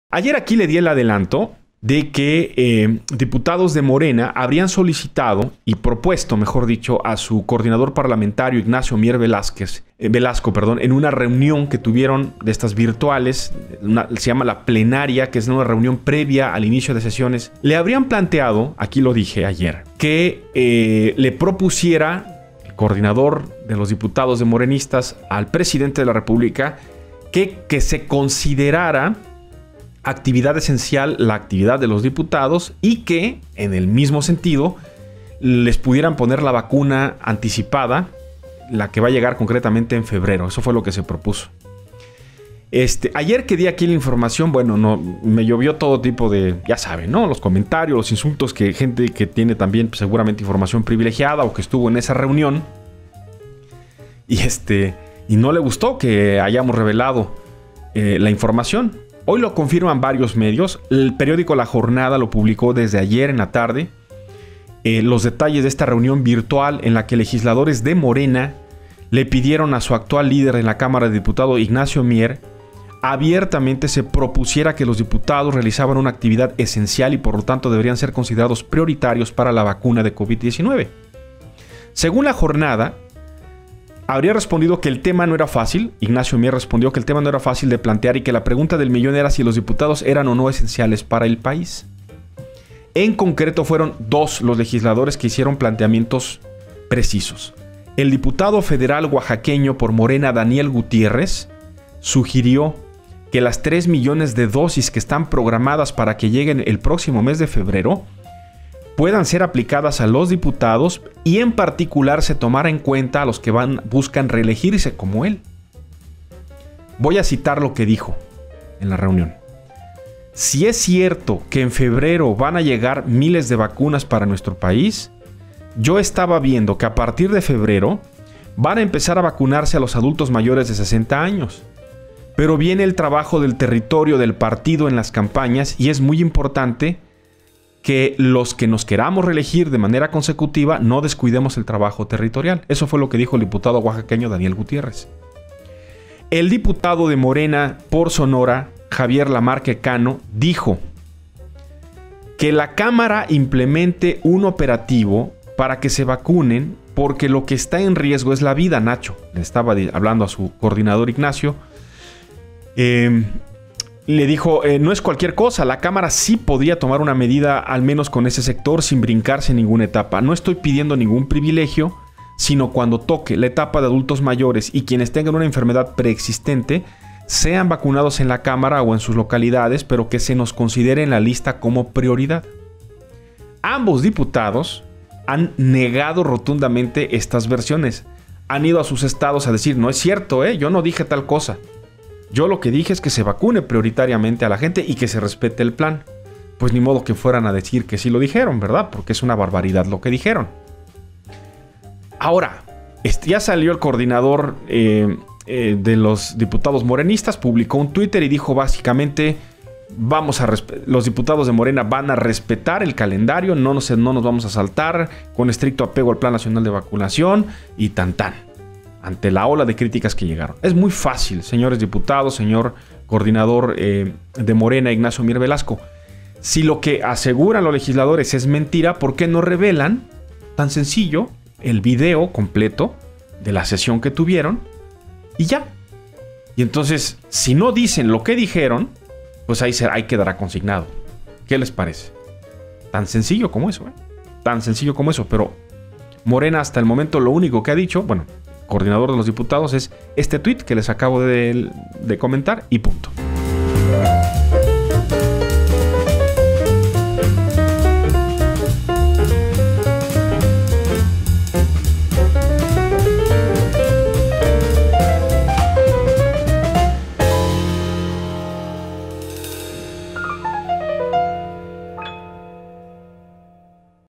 Diputados federales de Morena si pidieron ser vacunados para hacer campaña
Diputados de Morena pidieron ser prioridad en la vacunación contra COVID-19, por lo que realizaron un exhorto al coordinador del partido en la Cámara, Ignacio Mier Velasco.